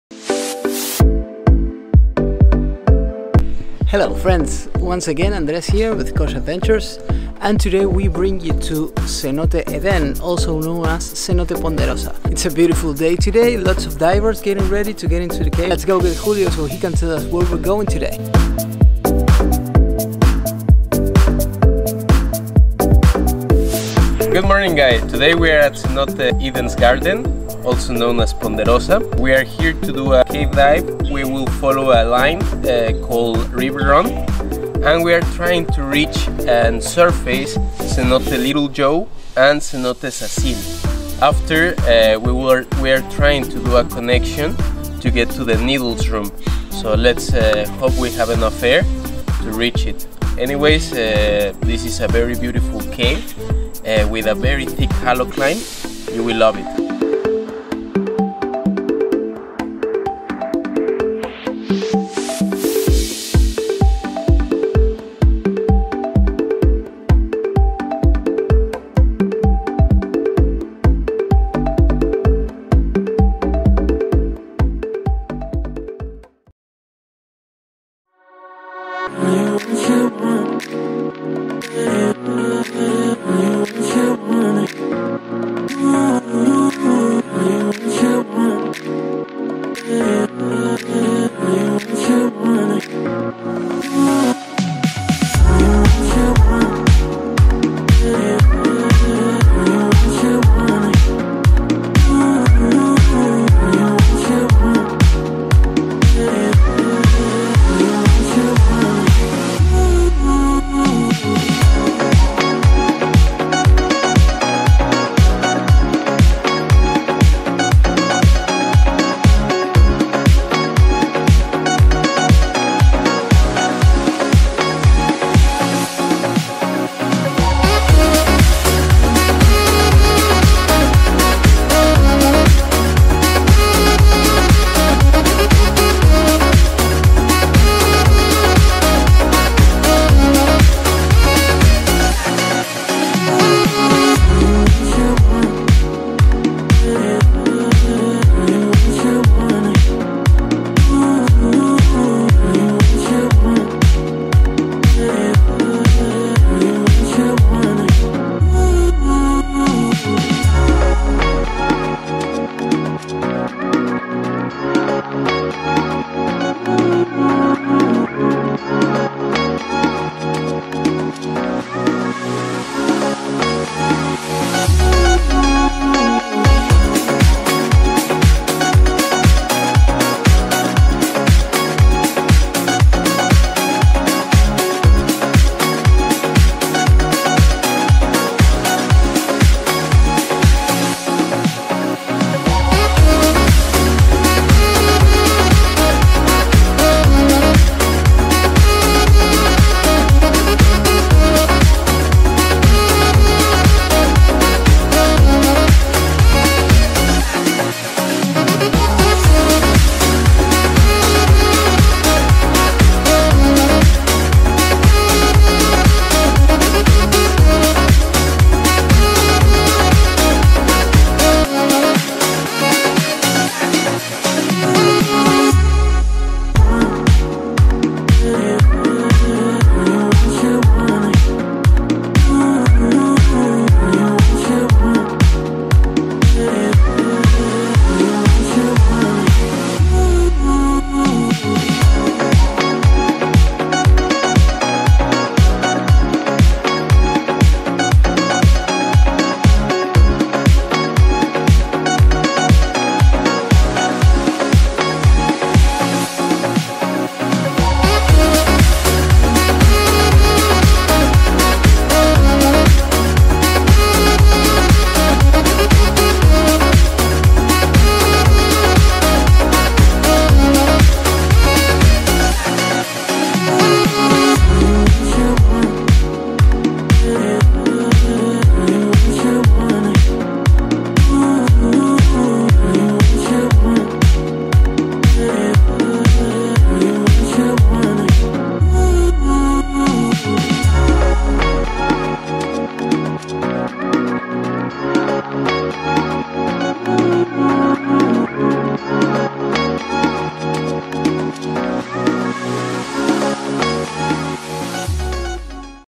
Cave divers go to Jardin del Eden cave AKA Ponderosa | Tulum, Mexico
This video is part of a series created for Ko'ox Adventures where cave diver Julio Salas and me go and explore different flooded caves in the Tulum area.

Video created for Ko'ox Adventures